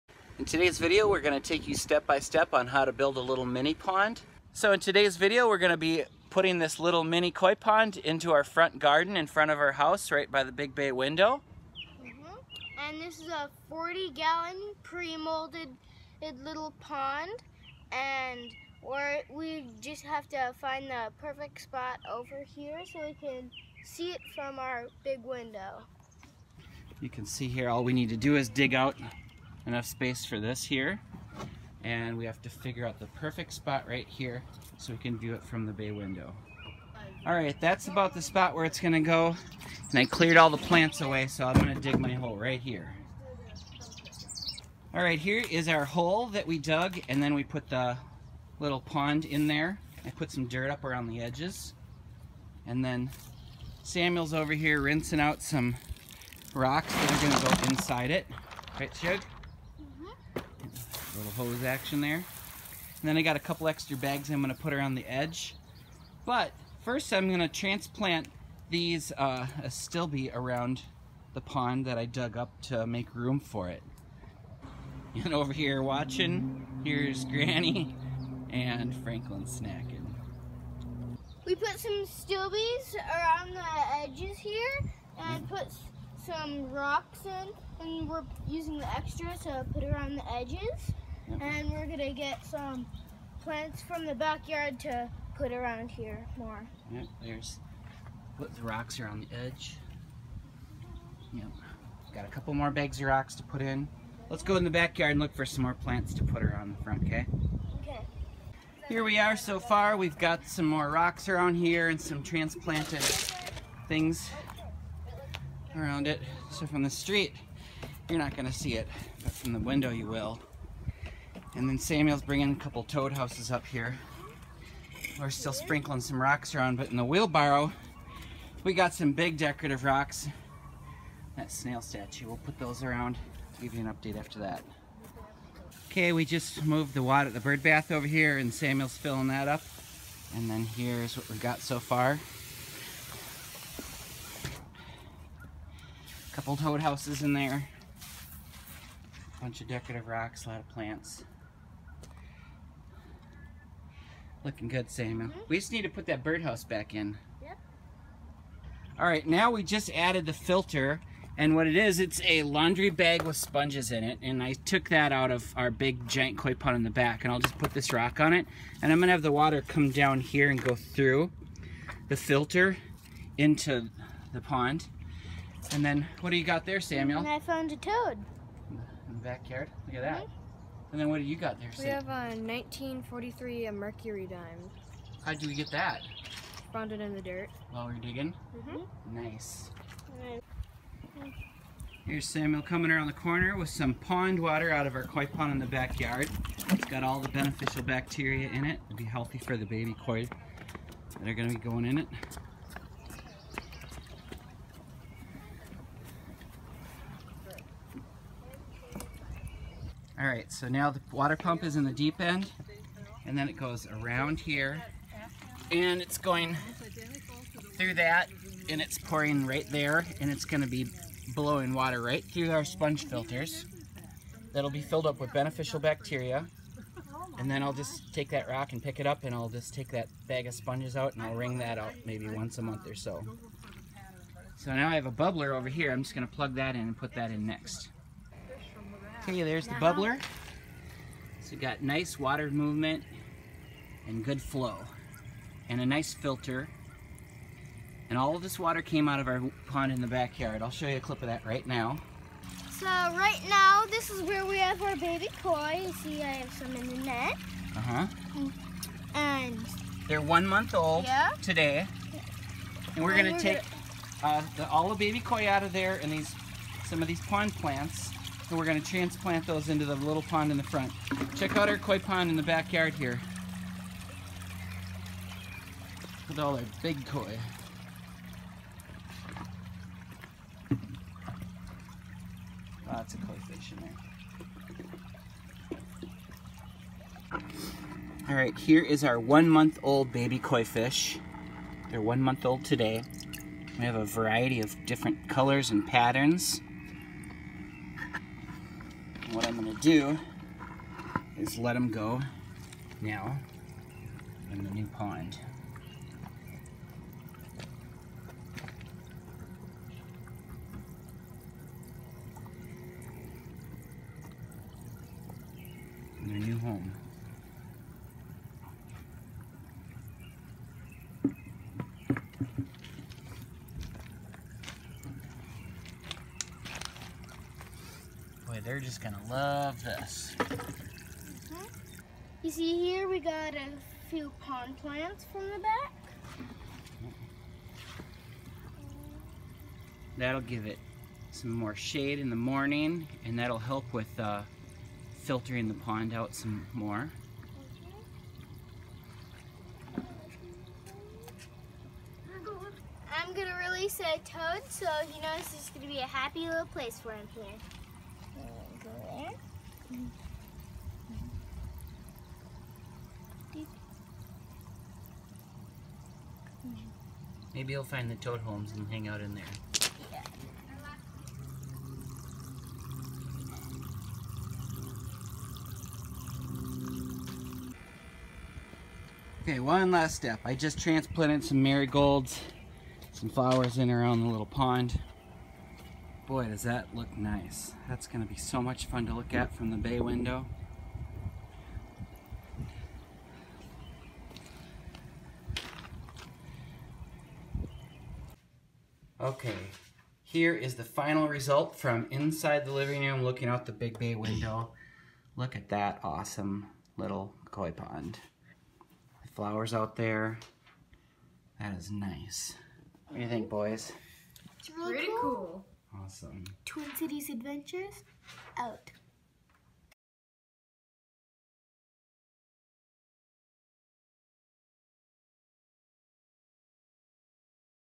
Mini Pond build ~ With 4 Week Old Koi Fish ~ Part 4 ~ Minnesota Koi pond
This video is about Mini Pond build ~ With 1 Month Old Koi Fish